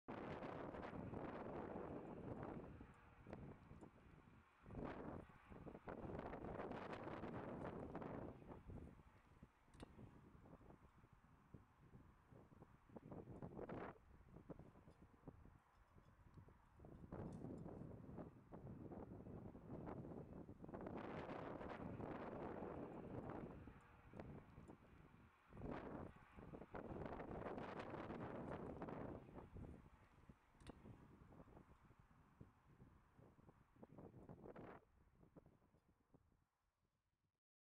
low flying chopper
today a bizarre thing happened - this helicopter flew really low - could almost see the pilots face. He semed to be following the electricity pylons.